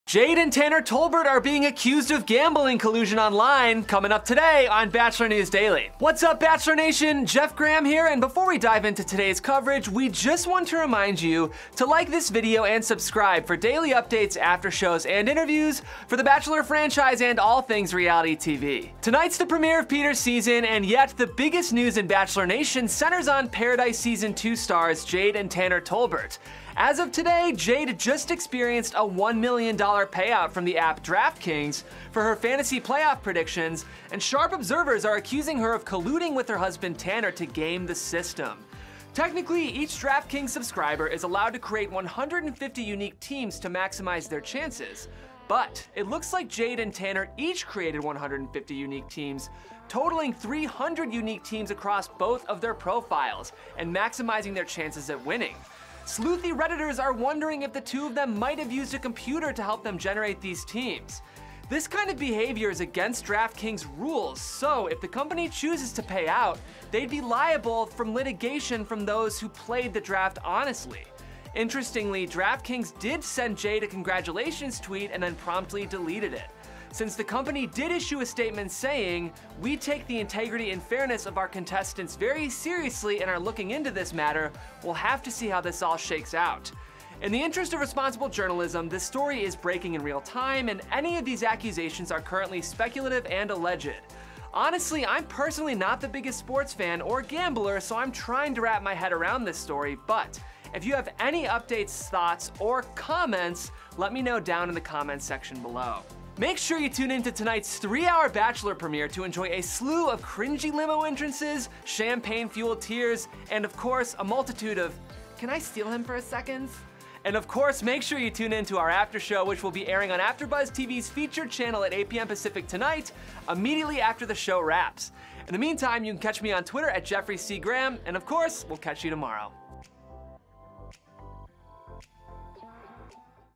Jade and Tanner Tolbert MILLION DOLLAR DraftKings Collusion EXPLAINED
Jade and Tanner Tolbert are being accused of gambling collusion online, coming up today on Bachelor News Daily with Jeff Graham (@jeffreycranegraham)!